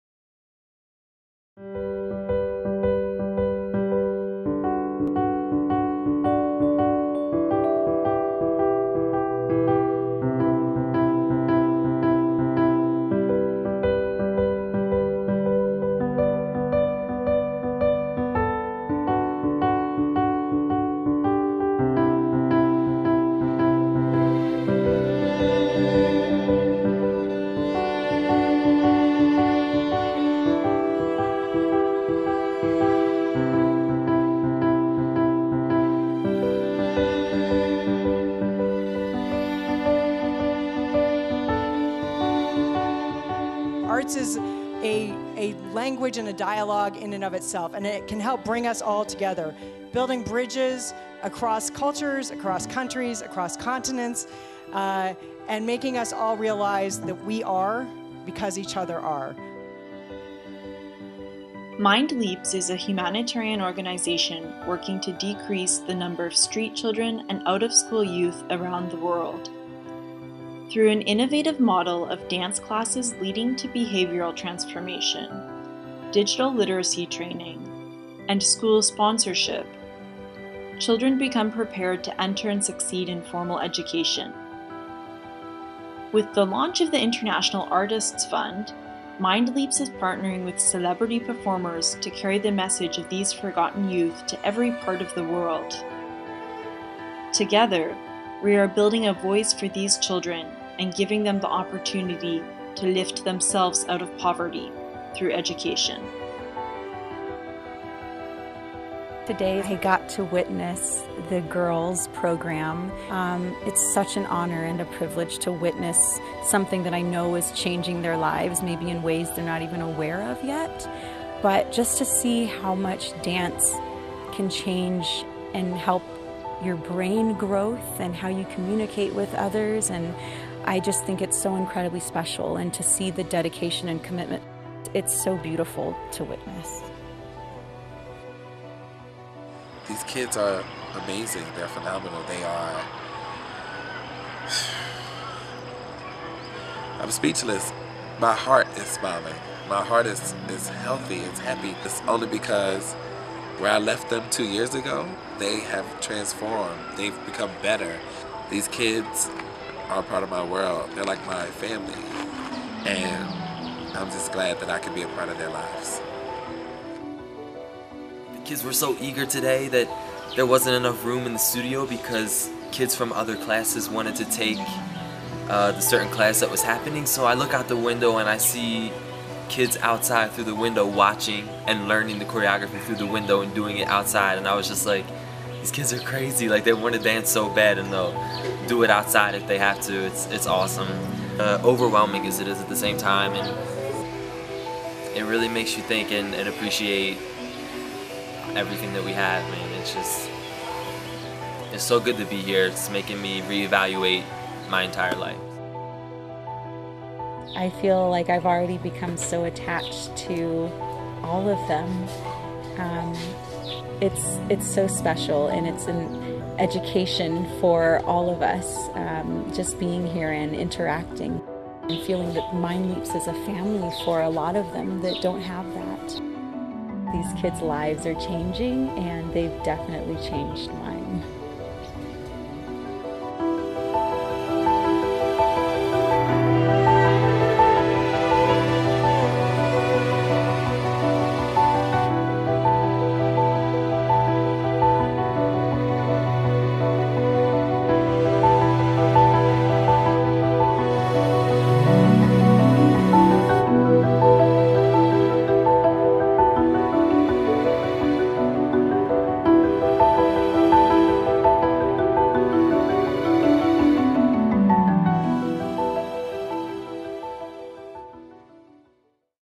MindLeaps Artistic Ambassadors with Misty Copeland
MindLeaps Artistic Ambassadors is a platform for celebrity performers to bring awareness to the needs of vulnerable youth. Artists travel to the MindLeaps' programs where they see in practice how dance is being used to attract youth off the streets and channel them into education.

From November 12 - 19, 2015, Misty Copeland traveled to MindLeaps' program in Rwanda.